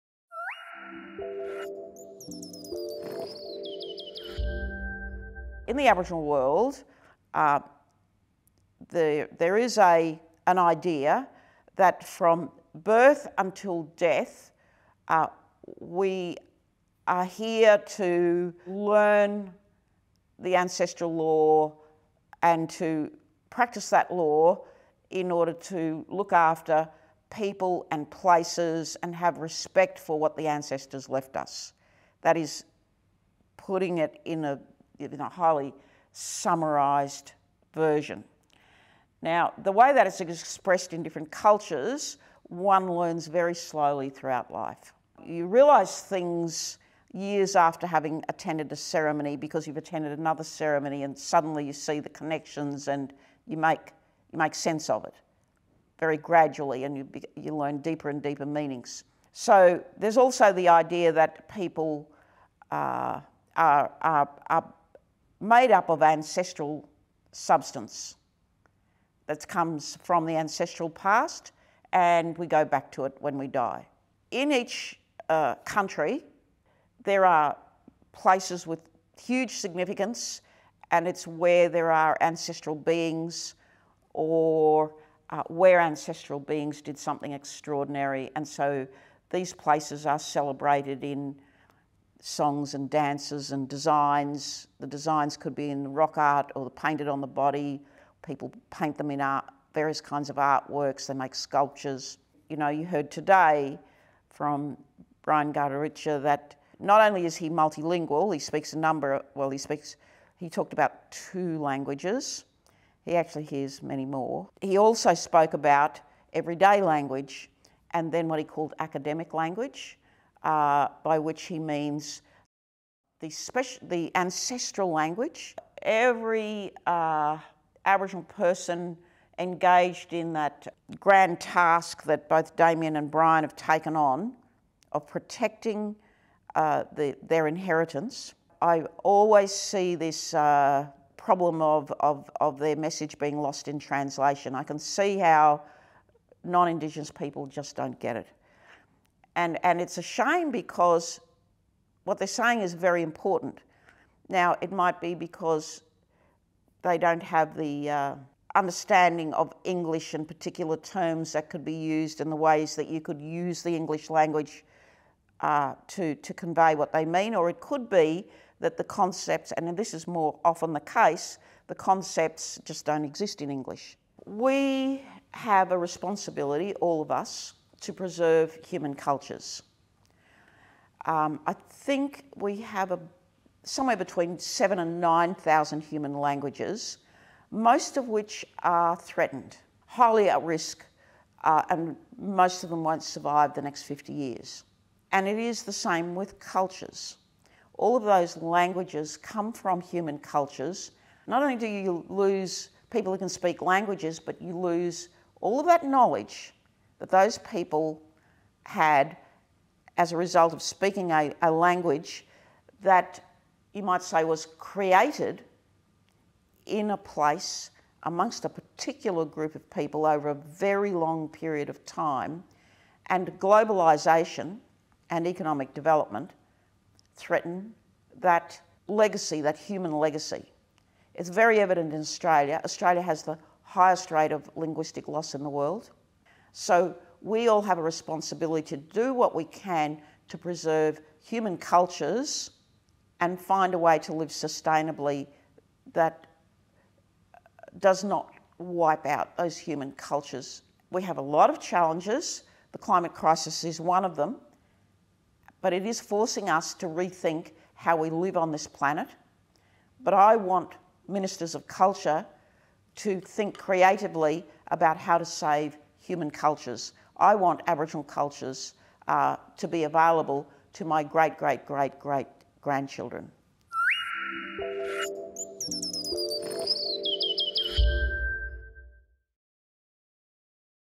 Dr Marcia Langton AO, Australia/UK Cultural Leadership Dialogue 2022
In August and October 2022, the Edinburgh International Culture Summit presented a new bilateral initiative in partnership with the Australia Council for the Arts, the Australian High Commission, London, British Council, Scottish Parliament, and the University of Melbourne.

This Cultural Leadership Dialogue was presented as part of the Australian government and British Council’s extensive and ambitious season of cultural exchange in 2021 and 2022, with twin events hosted at the Scottish Parliament, Edinburgh on 29 August 2022 and the University of Melbourne on 6 & 7 October 2022.